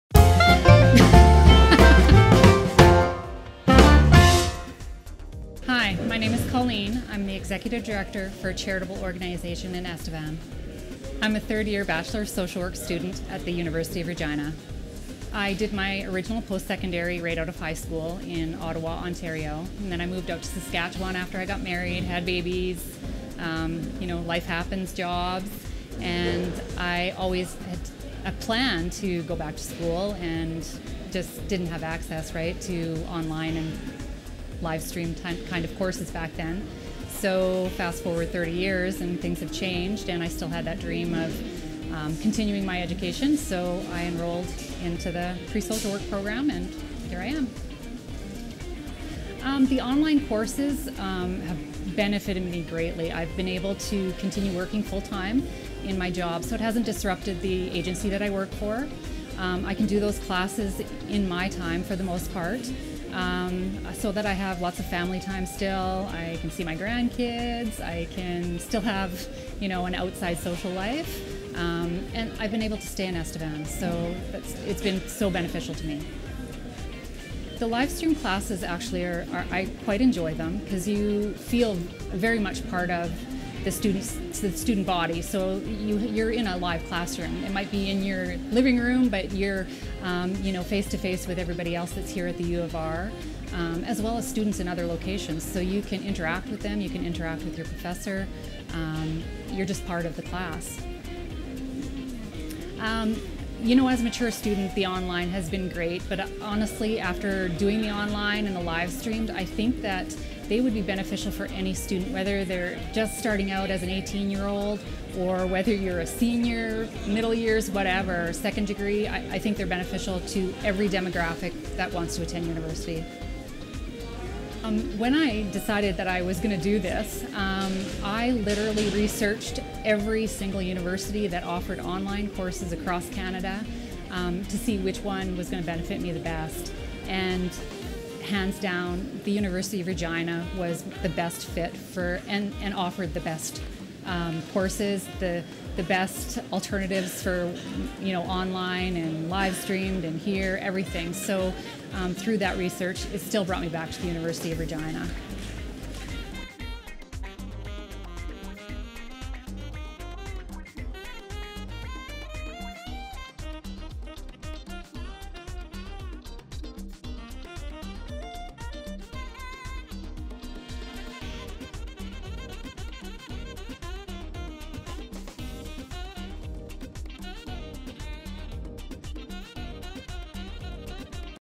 Colleen's Story - Distance Learning
Colleen talks about how distance learning options helped her to achieve her goal of a Bachelor of Social Work degree.